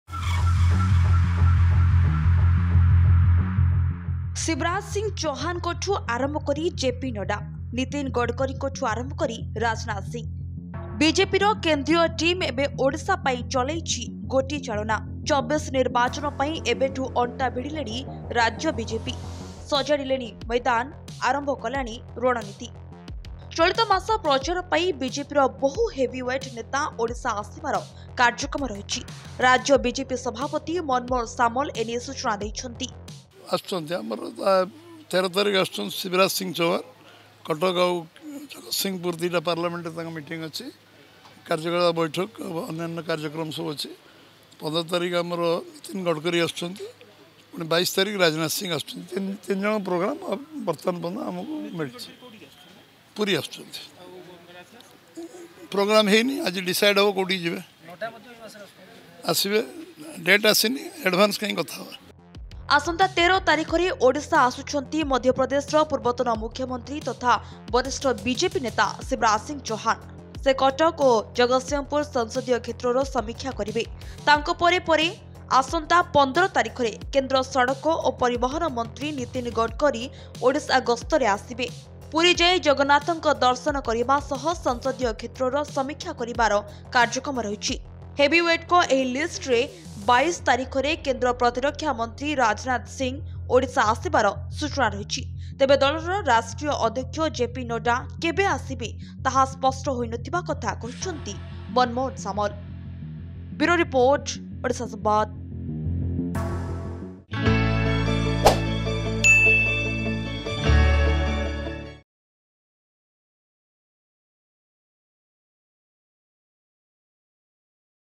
BJP 's Heavyweight Leaders Will Visit To Odisha
ଶିବରାଜ ସିଂ ଚୌହାନଙ୍କ ଠୁ ଆରମ୍ଭ କରି ଜେ ପି ନଡ୍ଡା।ନୀତିନ ଗଡ଼କରୀଙ୍କ ଠୁ ରାଜନାଥ ସିଂ। ବିଜେପିର କେନ୍ଦ୍ରୀୟ ଟିମ୍ ଏବେ ଓଡ଼ିଶା ପାଇଁ ଚଲେଇଛି ଗୋଟି ଚାଳନା। ୨୪ ନିର୍ବାଚନ ପାଇଁ ଏବେଠୁ ଅଣ୍ଟା ଭିଡିଲାଣି ରାଜ୍ୟ ବିଜେପି । ସଜାଡ଼ିଲଣି ମଇଦାନ, ଆରମ୍ଭ କଲାଣି ରଣନୀତି।